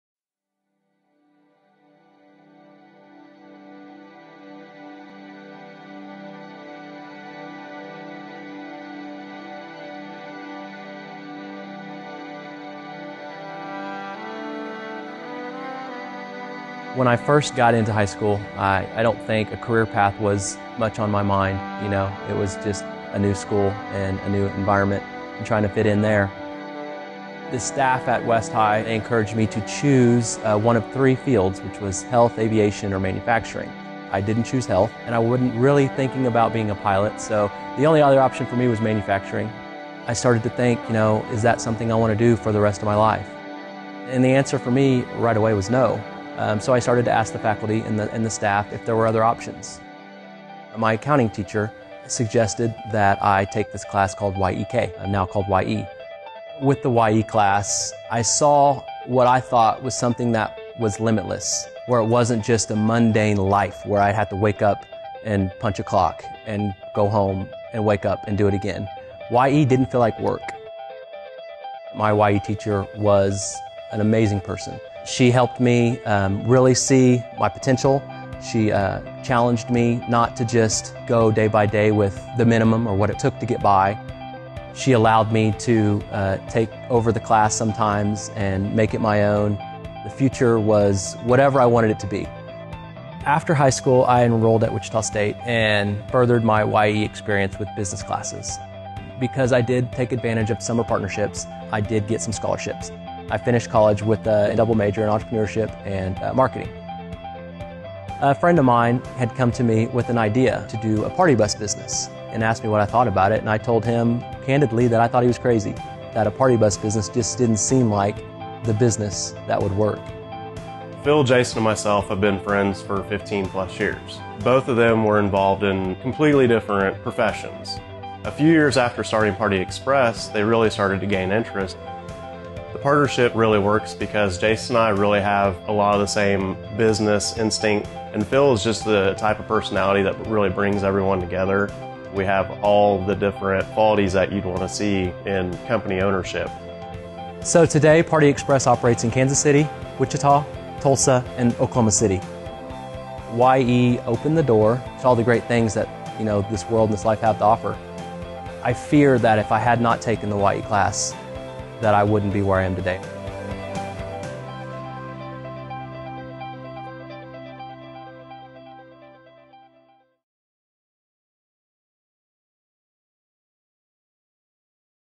"Jason Bayless" - We Are Koch, by Koch Industries
When presented with limiting pathways from his high school, Jason took matters into his own hands and enrolled in Youth Entrepreneurs. Fifteen years later, that ambition has transformed his glimpse at a better life into a reality.